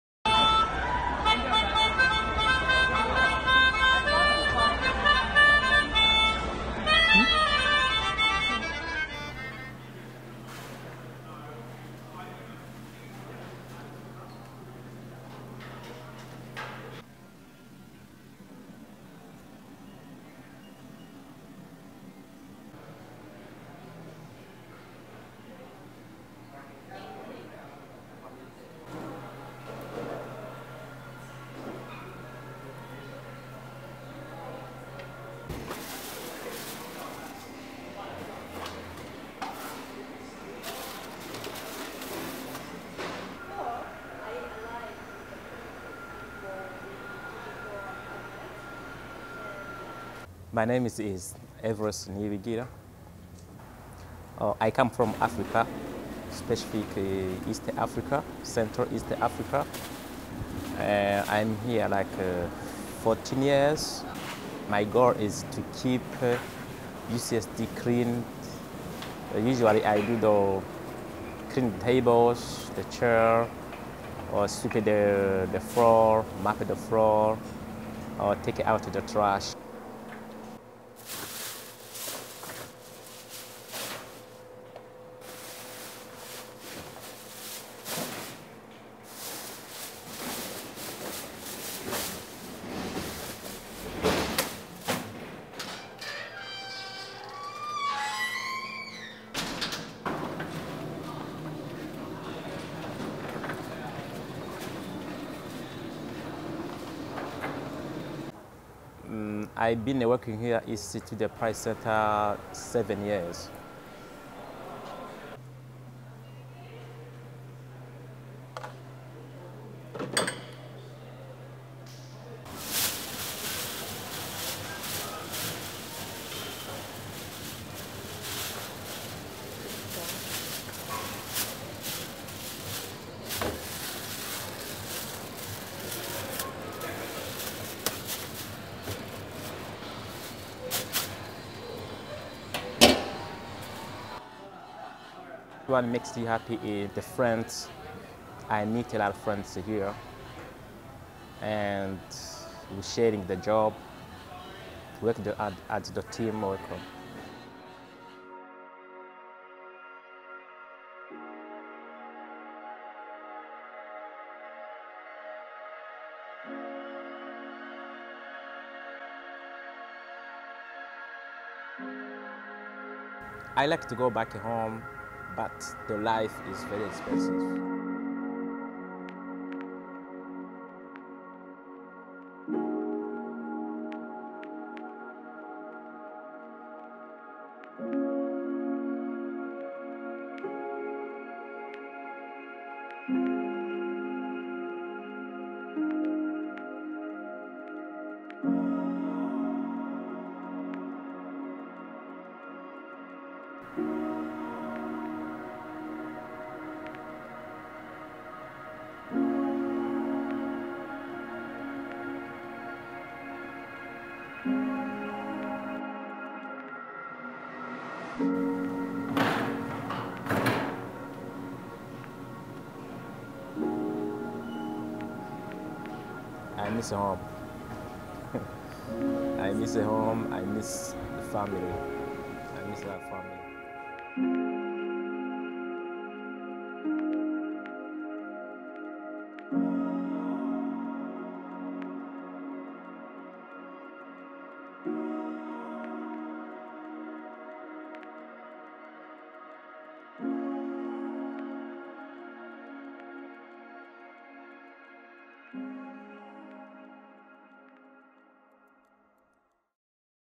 The People's Custodian - A VIS 70N Project
This was a video project for my VIS 70N class last year that I finally decided to publish onto here. I hope you all enjoy the story behind this and take away something from this video. Thank you to my partners and also to the Professor and TA !